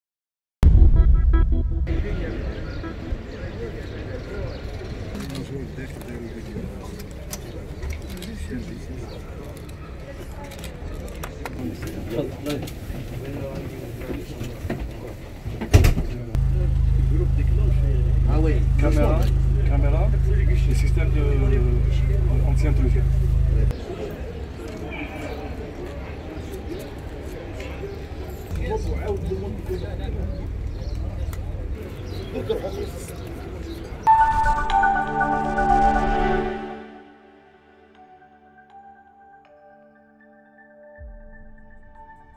تبسة : وضع مكتب بريدي متنقل حيز الخدمة
تبسة (وأج) - تم  بولاية تبسة وضع أول مكتب بريدي متنقل حيز الخدمة لفائدة زبائن بريد الجزائر وذلك في إطار التدابير الوقائية المتخذة ضد تفشي فيروس كورونا المستجد "كوفيد-19"